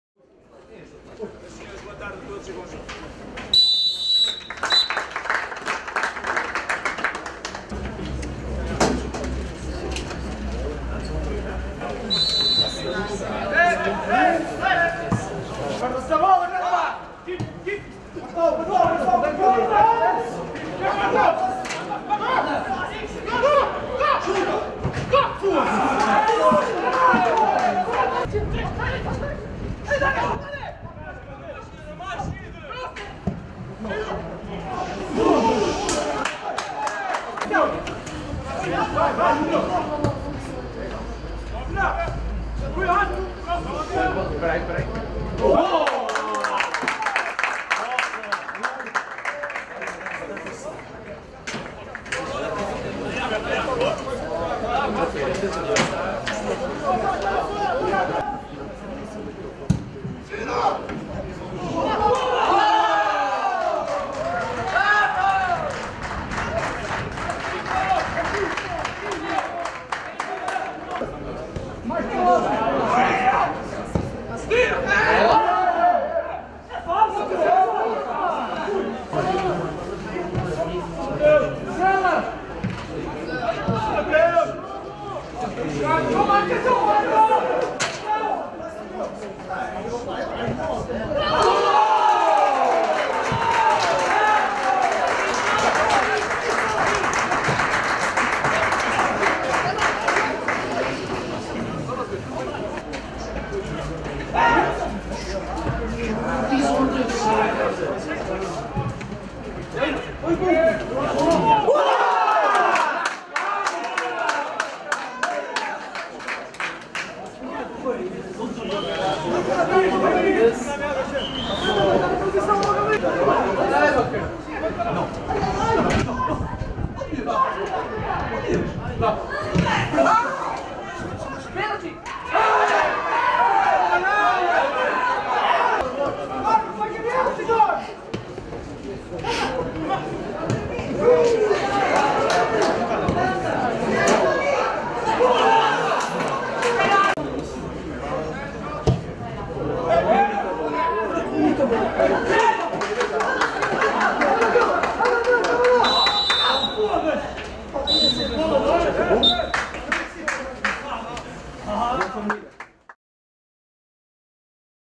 CD Feirense 2-2 FC Famalicão (Liga Revelação)
A equipa sub-23 do Futebol Clube Famalicão empatou a duas bolas frente ao Clube Desportivo Feirense em partida respeitante à 27.ª jornada da Liga Revelação.
O extremo Luka voltou a demonstrar a sua apurada apetência pelo golo, assinando um bis que valeu mais um ponto ao conjunto orientado por Vaz Pinto.